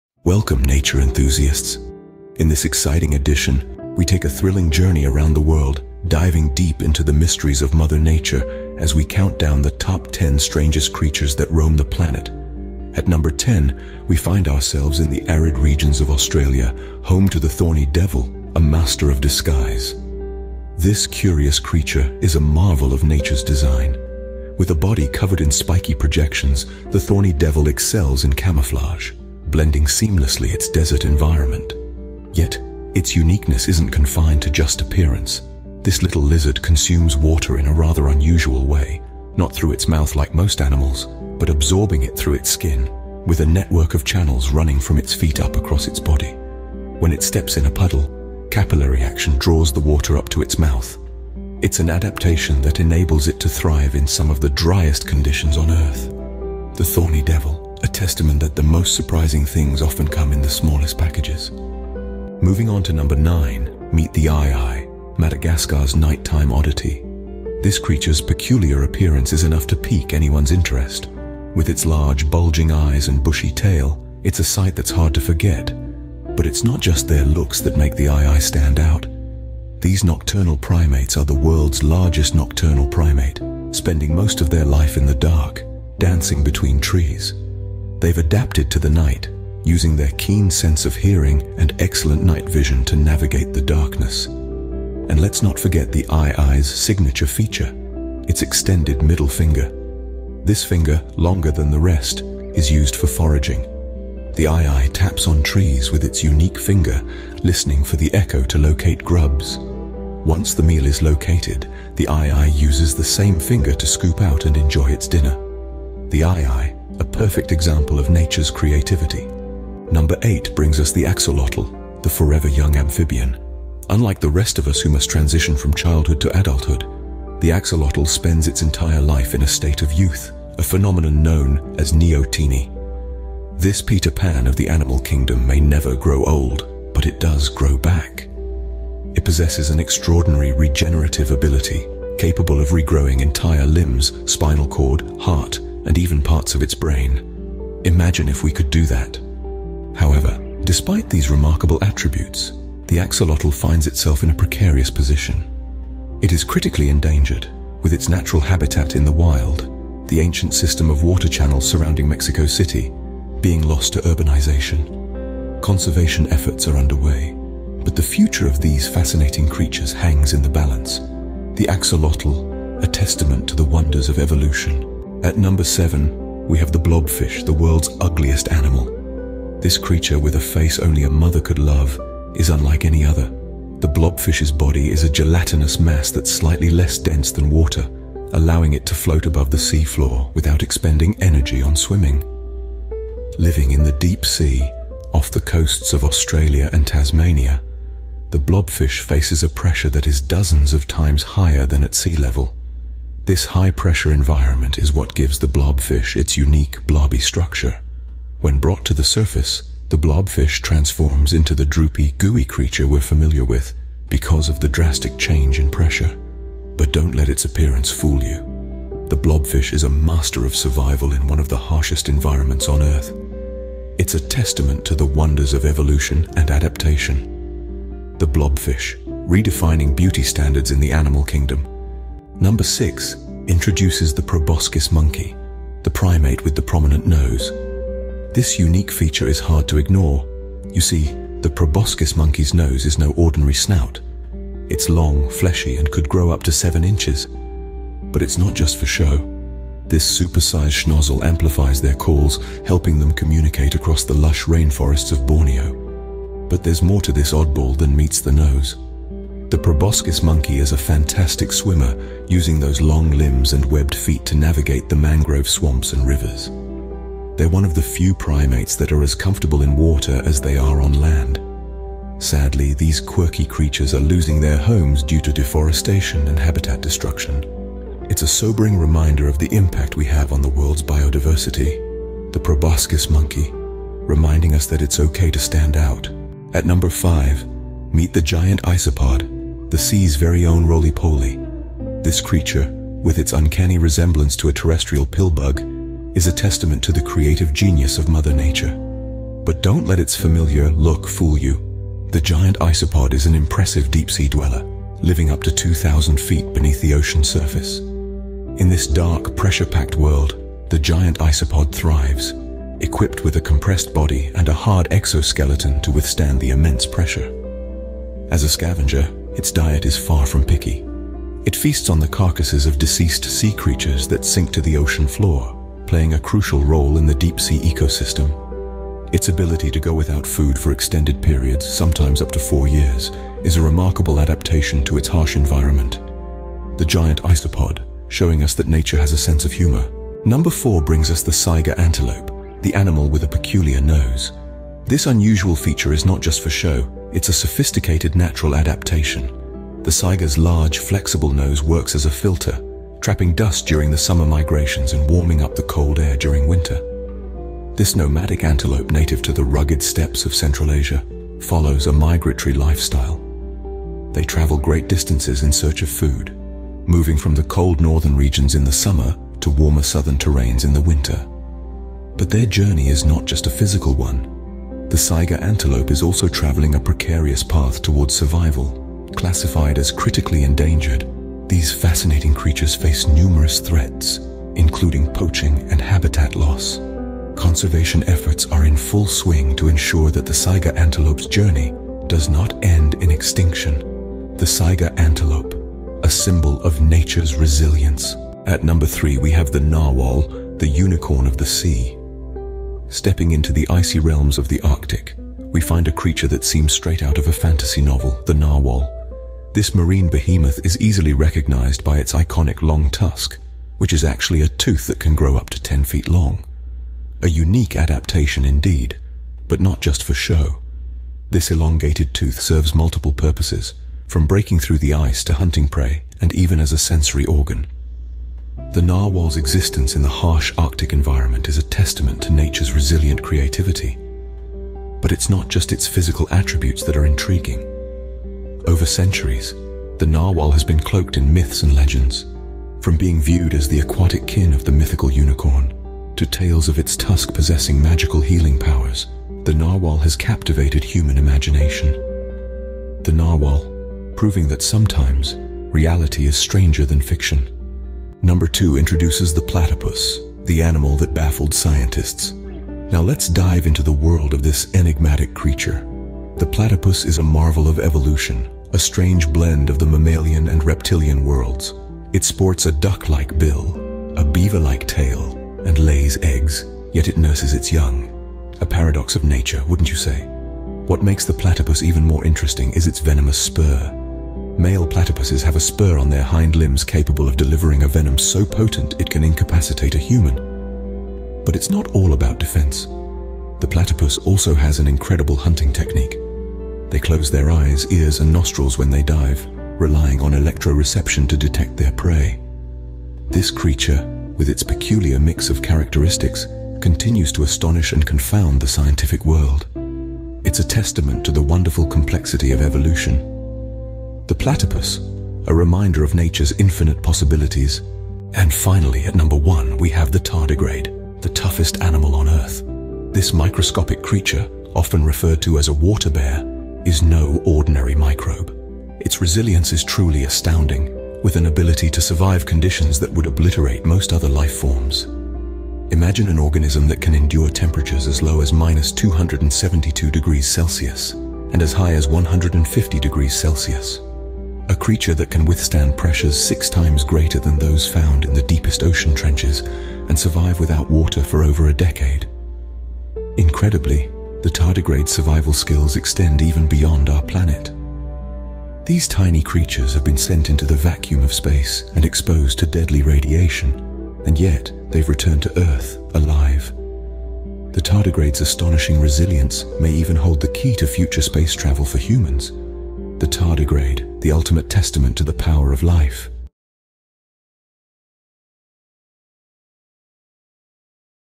Top 10 Strangest Creatures: A Journey into the Wild
Top 10 Strangest Creatures: A Journey into the Wild by finanzia's Workspace

OUTLINE:

00:00:00 The Thorny Devil
00:01:02 The Aye-Aye
00:01:59 The Axolotl
00:02:55 The Blobfish
00:03:52 The Proboscis Monkey
00:04:46 The Giant Isopod
00:05:40 The Saiga Antelope
00:06:38 The Narwhal
00:07:38 The Platypus
00:08:38 The Tardigrade